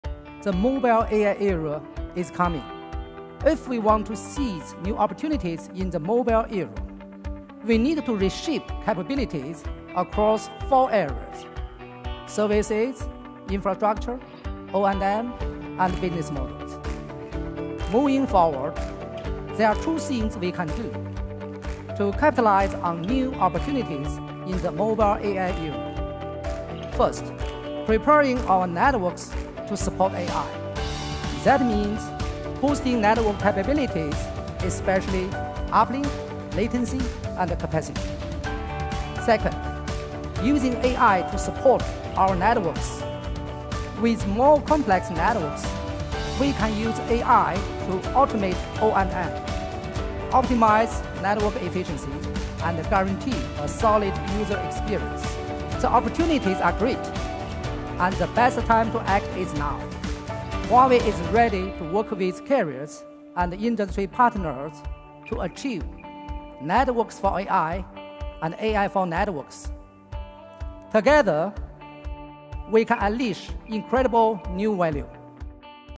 Maximizing New Growth Opportunities in the Mobile Al Era
At HWMBBF 2024, Huawei Corporate SVP and President of ICT Sales & Service Li Peng stated that the MobileAI era presents unlimited opportunities, and the wireless industry can share in the benefits if it fuses well with AI.